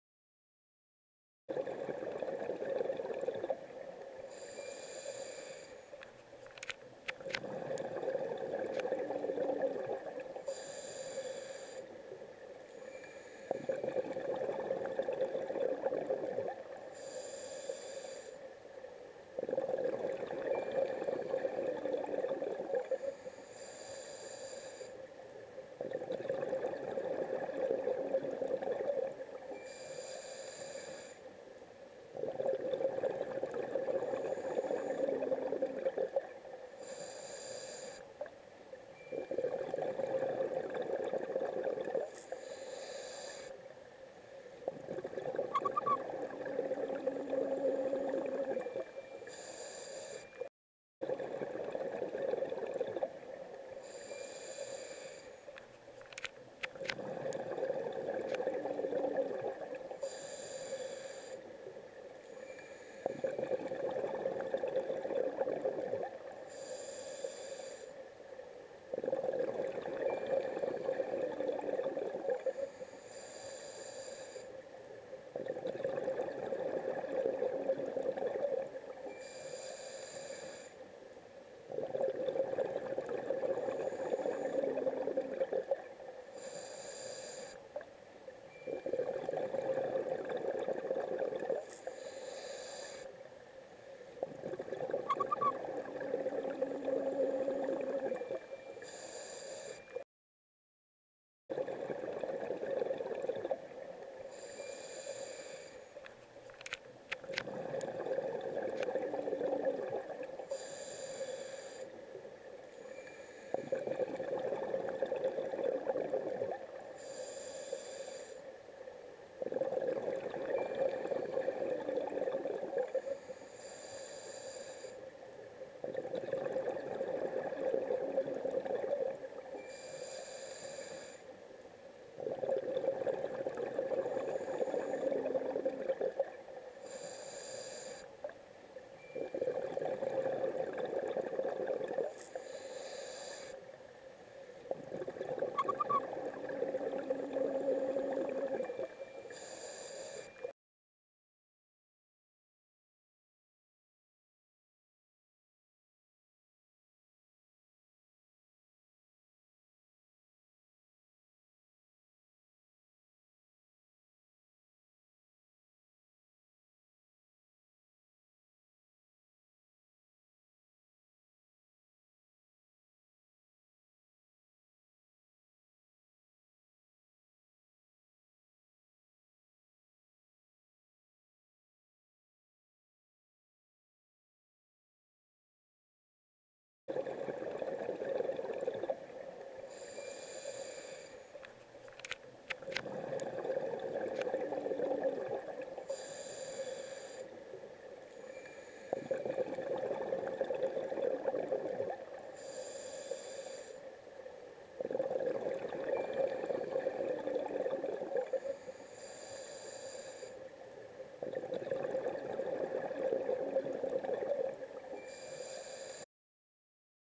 Belize Misc Footage 11-25-2017 to 12-02-2017
Misc. scuba diving footage from Belize.